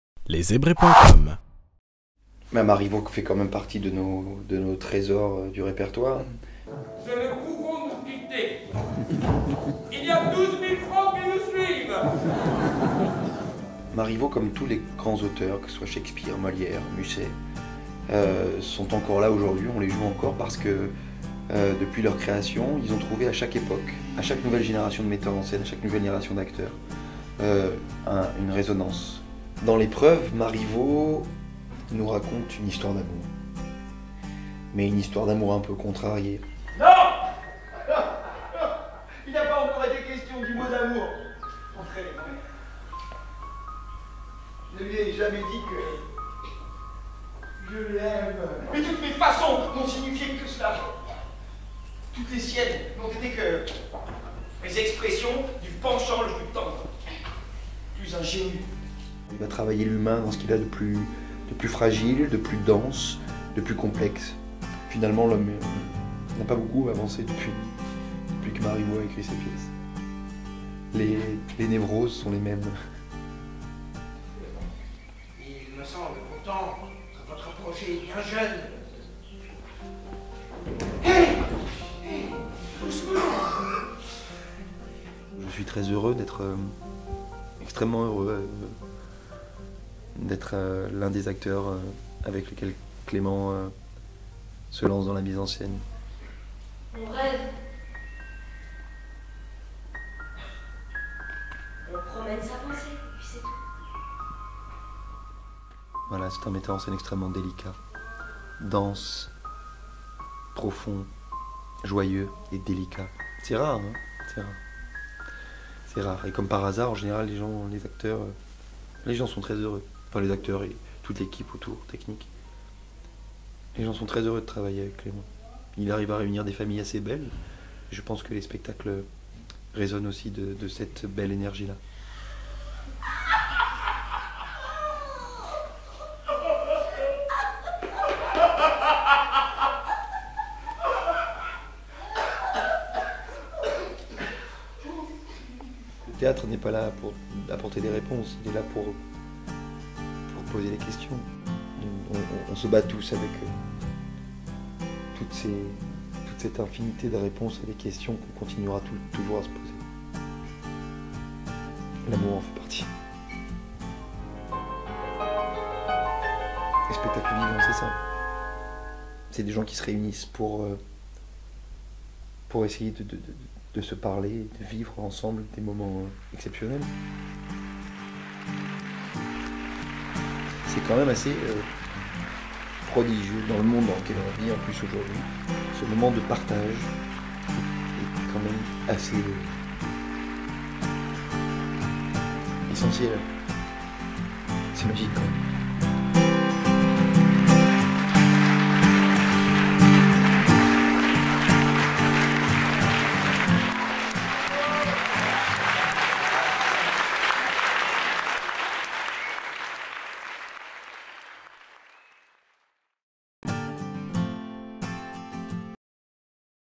Interview de Loïc Corbery (à l'affiche de L'Epreuve au Théâtre de l'Ouest Parisien)
Loïc Corbery, de la Comédie-Française, nous parle de L'Epreuve, de Marivaux, qu'il jouera jusqu'au 20 janvier 2013 au Théâtre de l'Ouest Parisien (Boulogne-Billancourt).